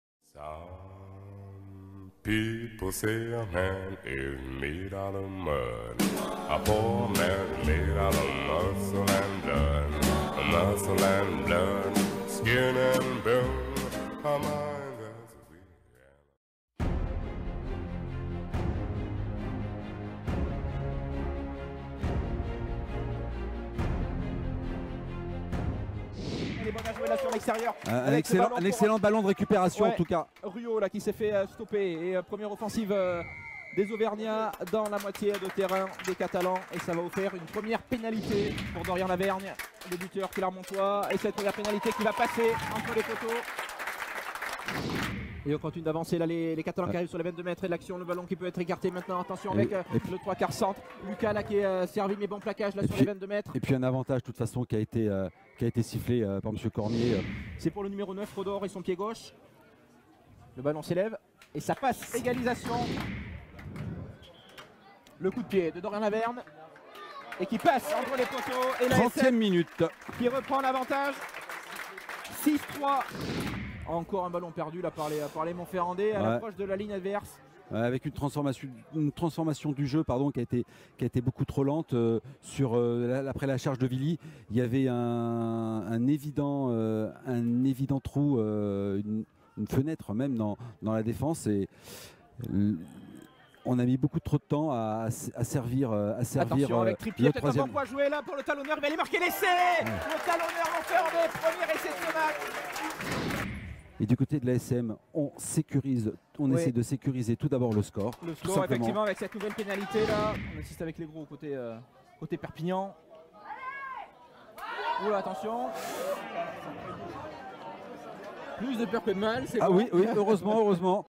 Championnat Espoir 2017 18 Match retour N°17 ASM vs USAP
Jour 17 Match retour du championnat espoirs 2017-18 opposant l'ASM à L'USAP
Victoire de l'ASM